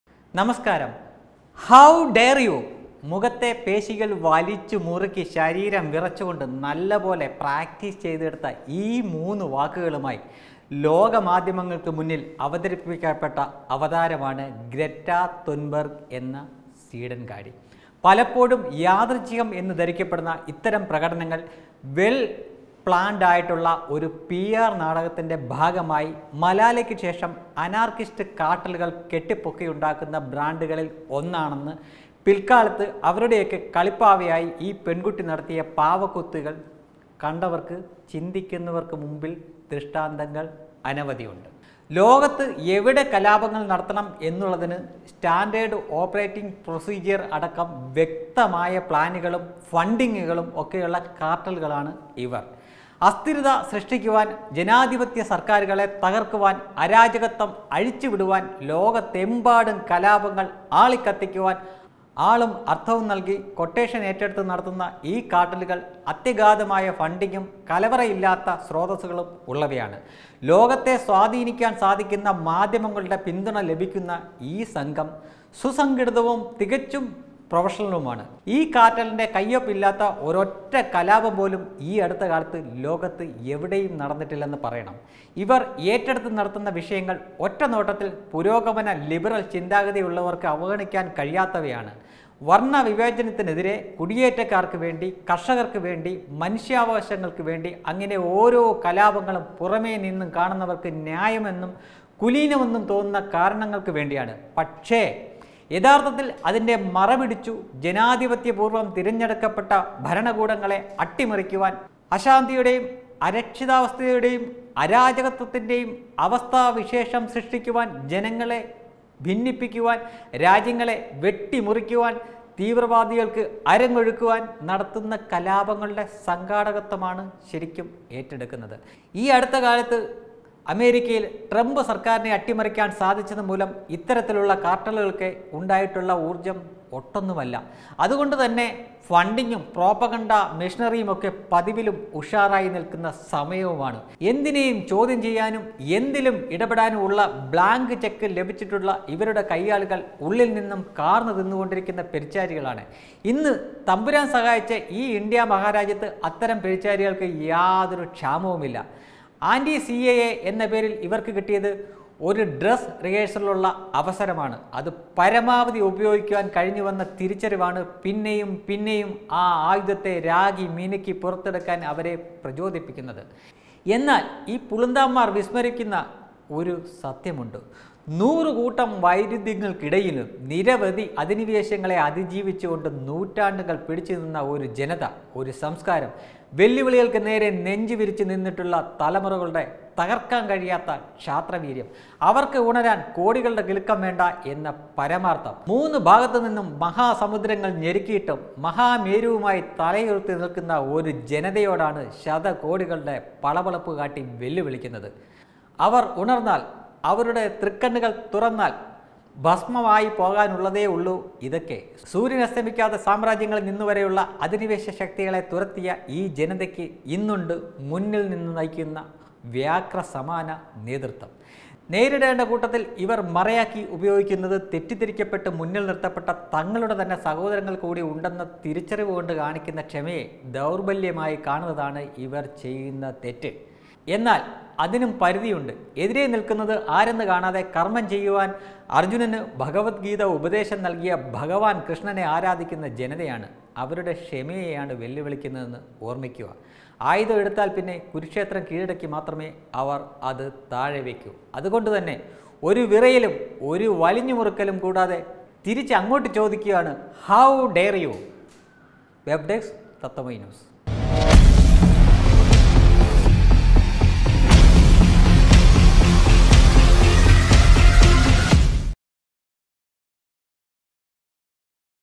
ഗ്രെറ്റ തൻബർഗ് എന്ന നാടകക്കാരിയും കൊറേ പ്രൊപ്പഗണ്ടക്കാരും | Greta Thunberg
ഗ്രെറ്റ തൻബർഗ് എന്ന നാടകക്കാരിയും കൊറേ പ്രൊപ്പഗണ്ടക്കാരും,
ഇത് രാജ്യം വേറെയാണ്, നിങ്ങളുടെ പരിപ്പ് ഇവിടെ വേവില്ല  | Greta Thunberg